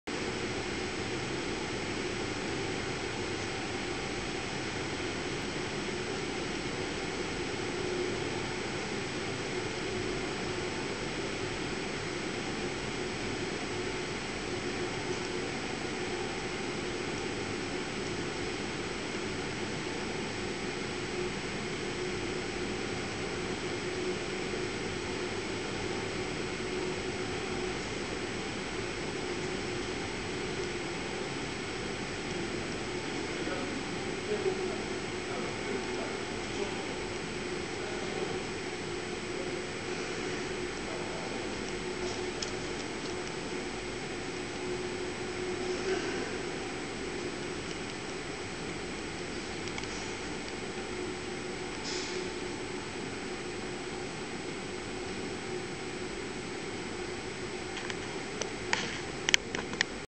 Shuttleworth Collection Old Warden Bedfordshire UK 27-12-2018 Hawker Sea Hurricane in for winter servicing.